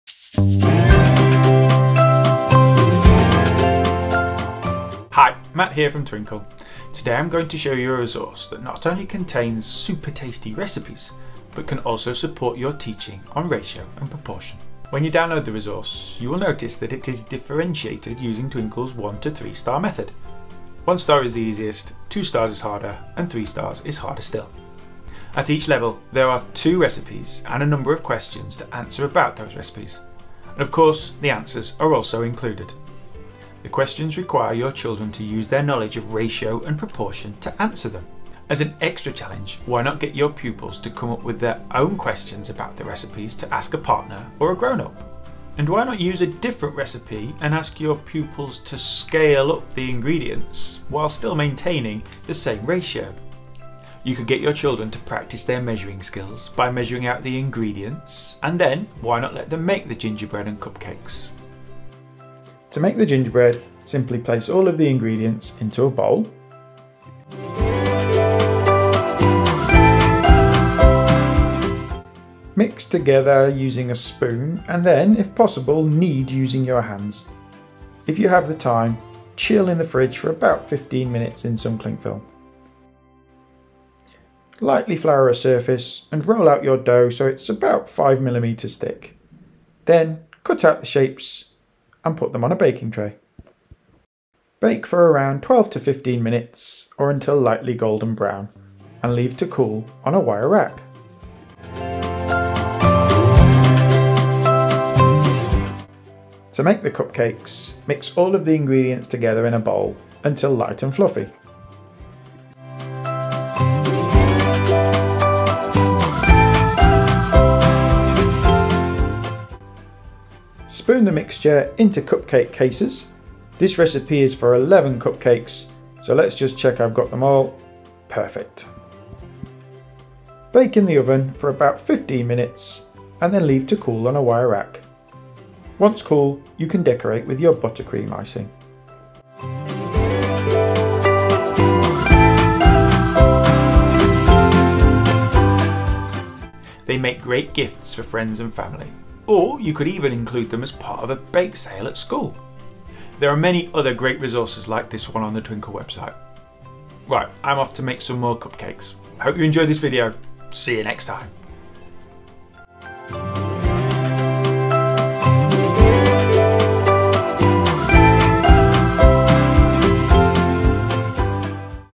Recipes Ratio and Proportion Worksheet
We hope you enjoyed learning about how to use the Recipes Ratio and Proportion Worksheet. This resource can be downloaded here:

In this video, Mat describes the different ways to use the Recipes Ratio and Proportion Worksheet.

The Twinkl team upload videos regularly Monday to Friday with tips for teachers, homework help guides and home learning support for parents.